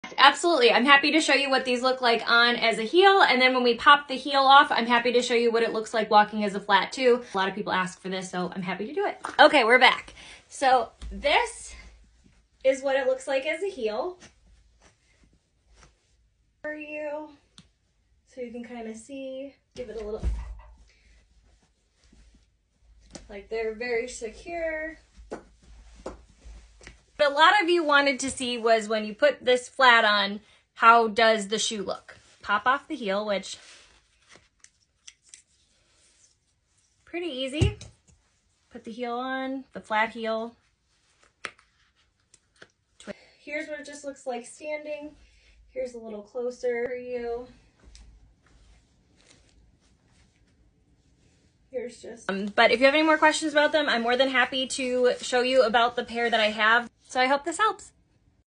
Review of Pashion shoe’s convertible heels to flats. Shoes. High heels. Wedding shoes.
High heels that convert to flats. 👠🥿